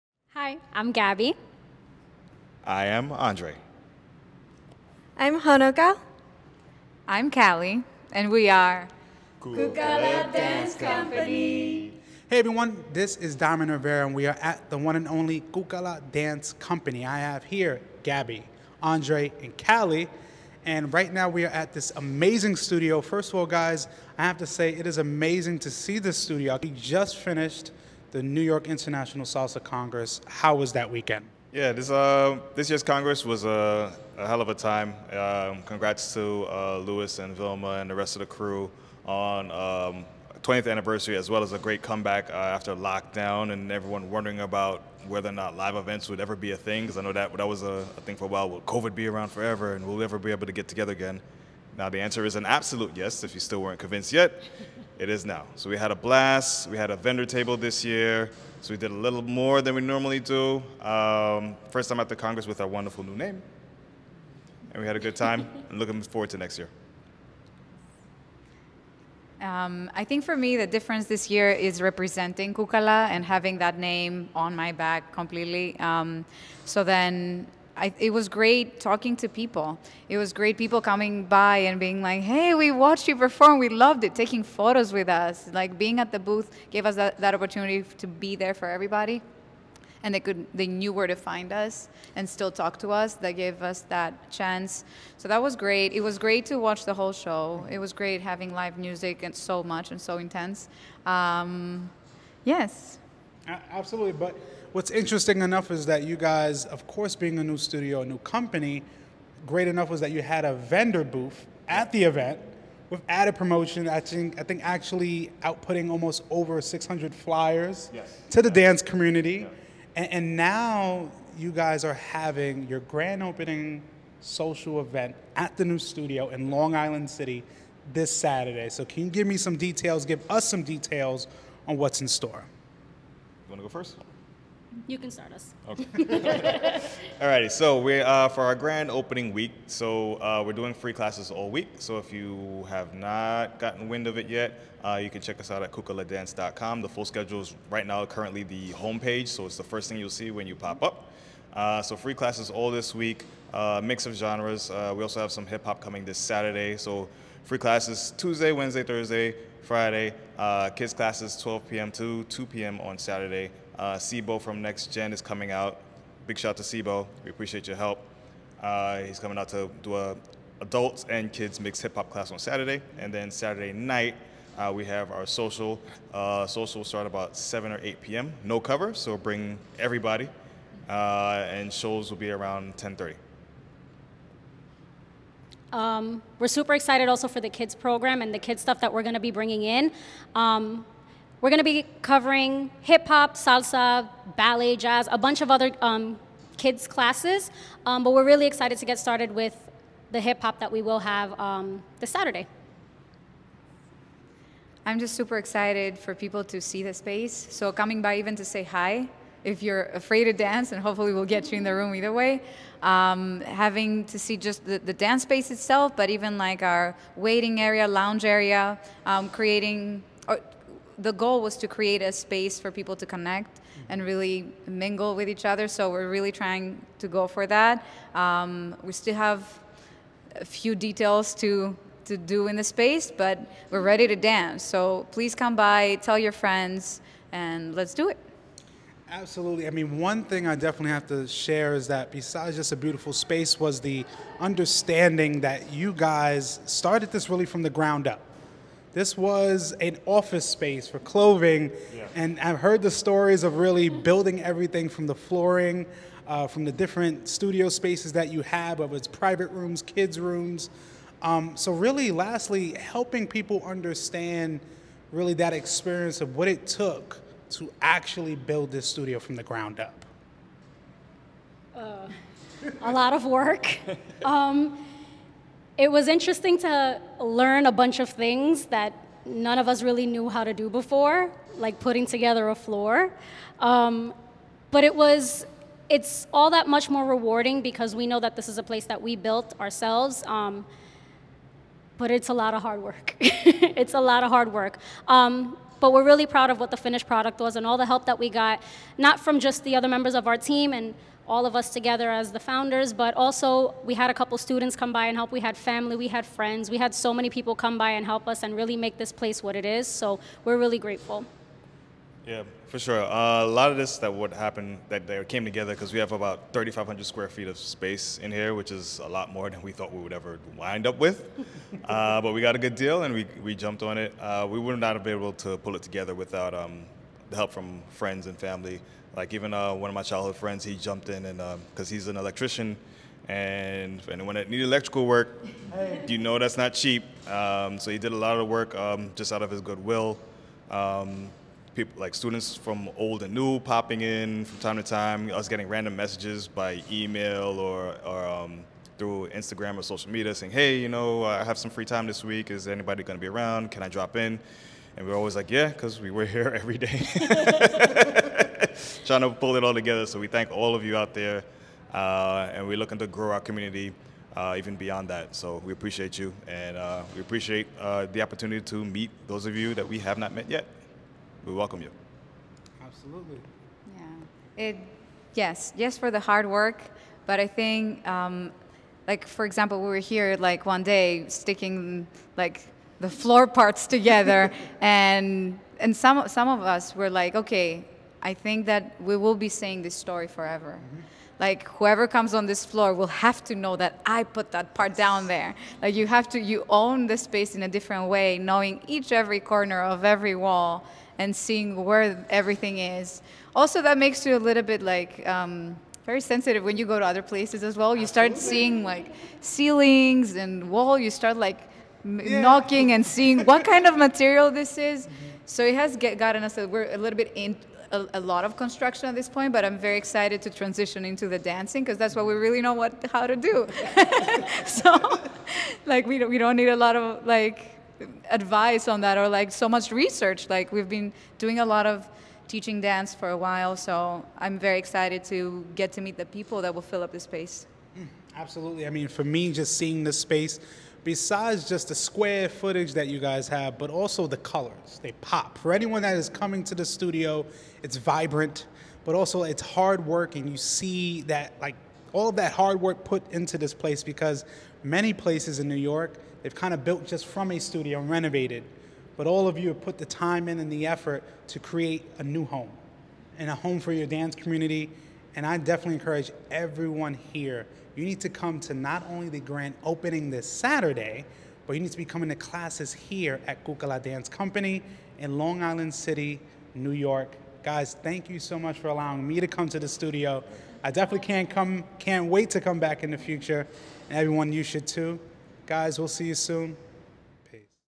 Talking With Cucala Dance Company
I went all the way to Long Island City, NY to visit Cucala Dance Company and recap everything new thats happening at the studio. Join them at their GRAND OPENING September 10th